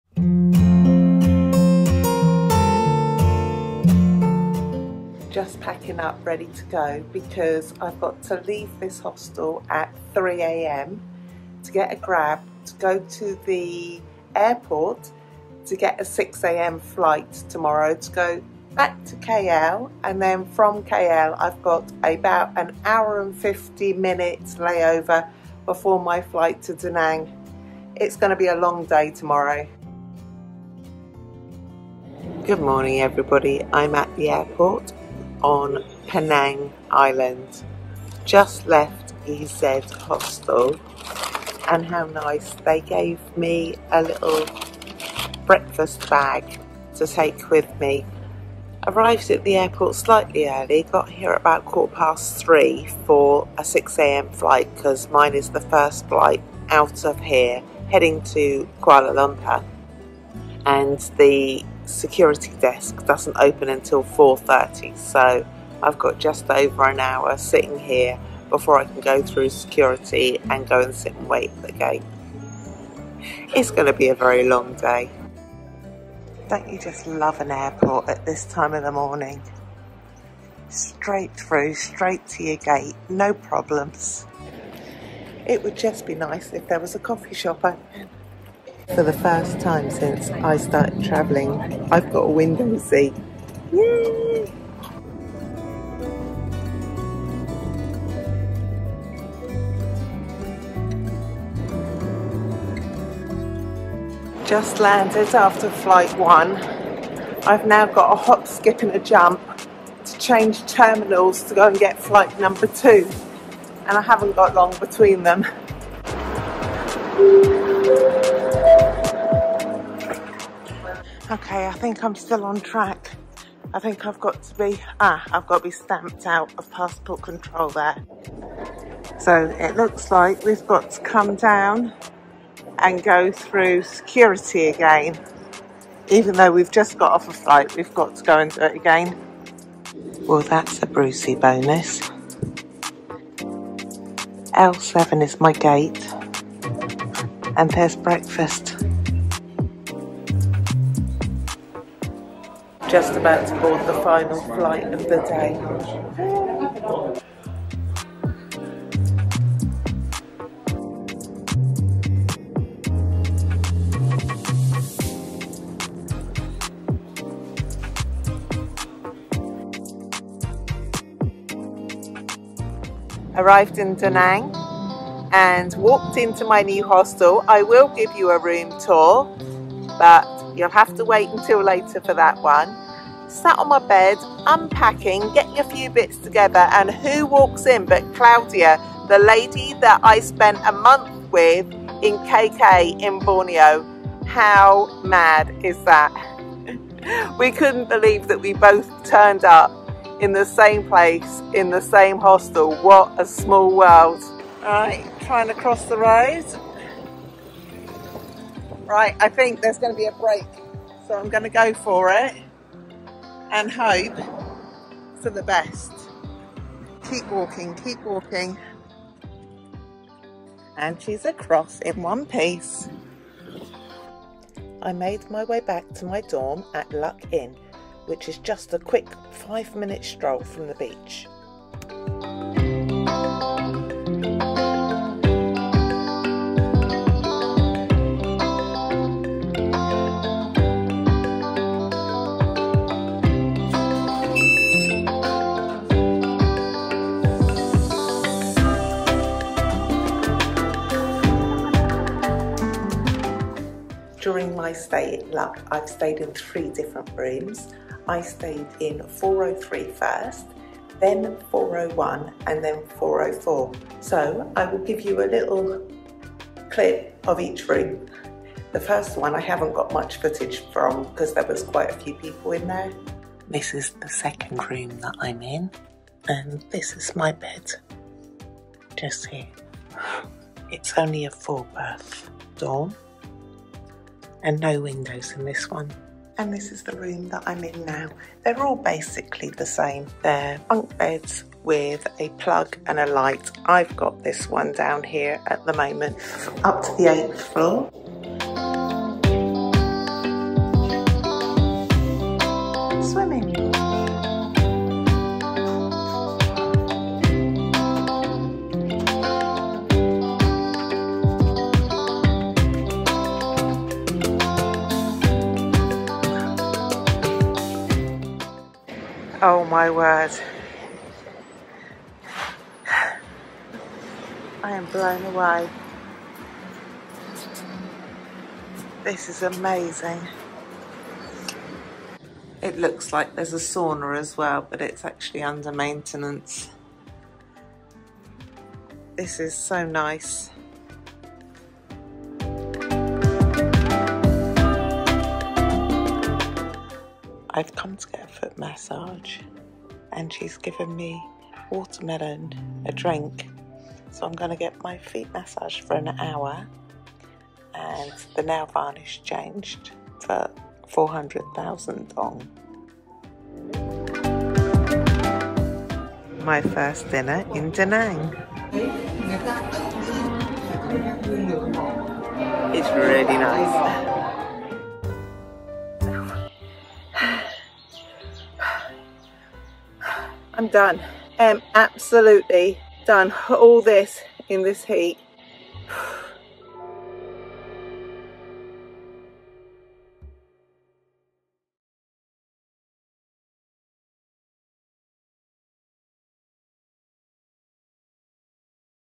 First time in Da Nang: Air Asia flights & my stay at Luk Inn
Join me on my flights from Penang to Kuala Lumpur and then to Da Nang, Vietnam! Have you been to Da Nang or stayed at Luk Inn? Share your experiences in the comments below!
This video captures my first time in Da Nang, and the surprise reunion with Claudia, who I spent a month with in KK Borneo. It's truly a small world! 🌏✈️🏨

1. AirAsia Flights: Watch as I travel from Penang to Kuala Lumpur and then hop on another AirAsia flight to Da Nang. I'll share my flight experiences.
2. Arrival in Da Nang: Touching down in Da Nang for the first time, I make my way to the city.
3. Staying at Luk Inn: Check out my cozy stay at Luk Inn hostel. From the comfortable rooms to the friendly staff, I'll give you a full tour of my new temporary home.
4. Reunion with Claudia: Unexpected reunion with Claudia, the lady I spent a month with in KK Borneo. We (off Camara) catch up and share our travel stories – proving that it's a small world after all!

Whether you're planning a trip to Da Nang or just love travel vlogs, this video is packed with tips, highlights, and a touch of serendipity. Don’t miss out!

0:00 - Introduction
1:10 - AirAsia Flight from Penang to Kuala Lumpur
3:45 - AirAsia Flight from Kuala Lumpur to Da Nang
6:20 - Arriving in Da Nang
7:50 - Checking into Luk Inn Hostel
9:30 - Reuniting with Claudia
11:50 - Exploring Da Nang & First Impressions
13:30 - Wrap Up and Final Thoughts